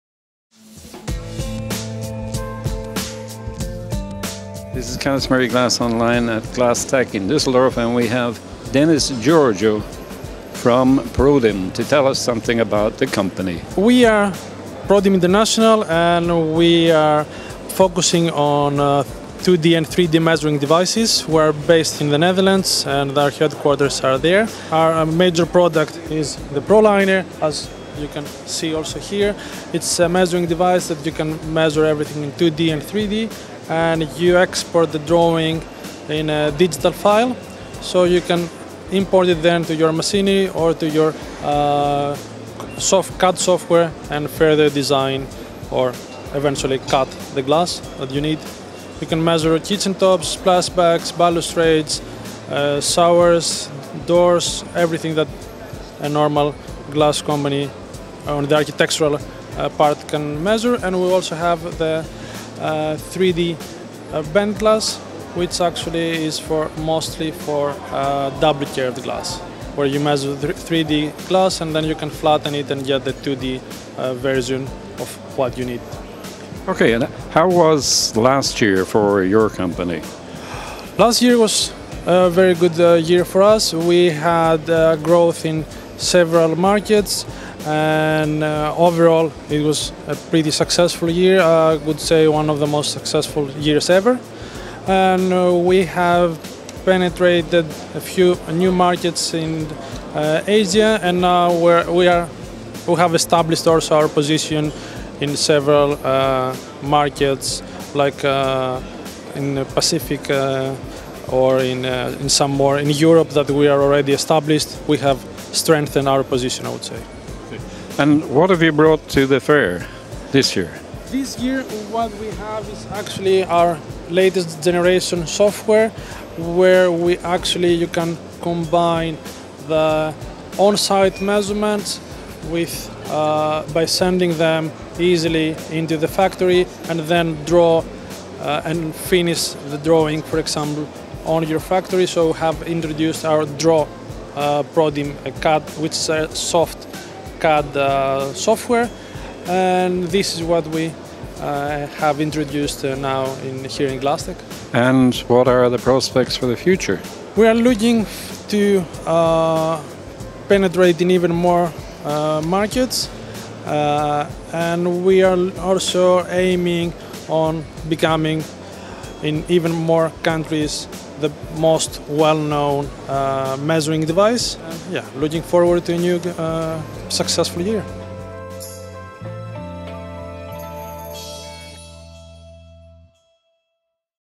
Prodim at glasstec with 2D and 3D measurement
Speaking to Dennis Georgiou, international sales at Prodim, we found out more about the Proliner, the company’s most famous product, and how it helps glassmakers to design and cut the glass to specifications.